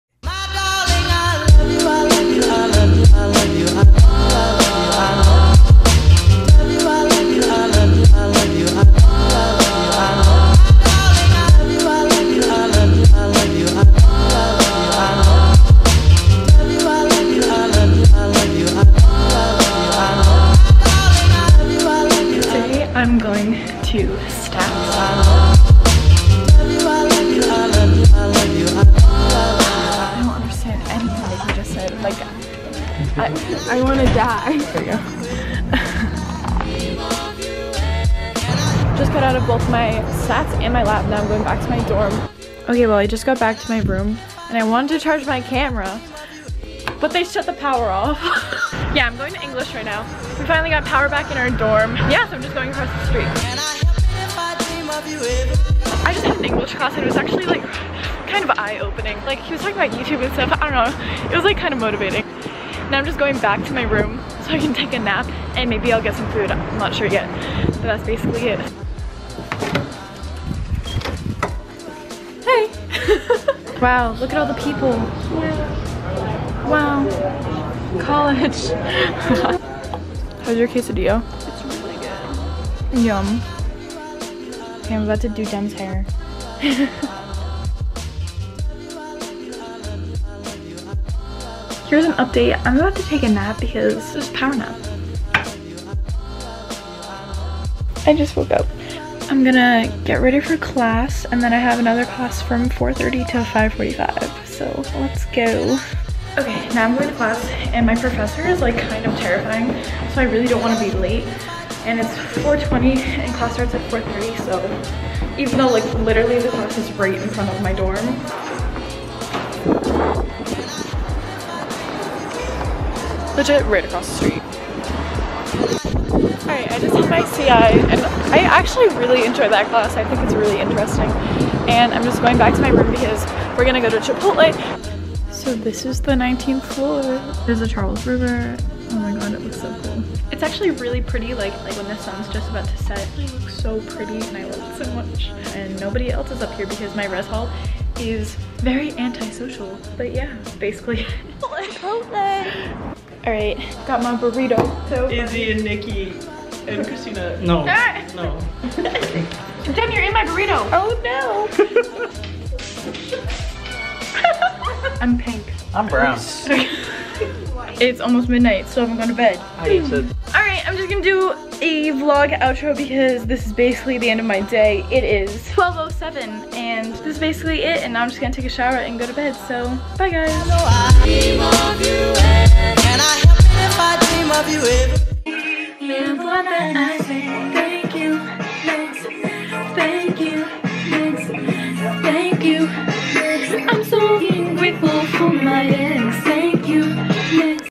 FIRST WEEK OF COLLEGE CLASSES | BOSTON | SECOND SEMESTER
HEY GUYS!! here is one day out of my first week of classes going back to school!! enjoy :)

《 WATCH IN 1080p FOR HIGH DEFINITION 》

lets be friends!! DM me!

Location: Boston, MA
Age: 18, freshman in college
Cameras:
Edit: imovie

In this video I show you my first week of college classes in boston!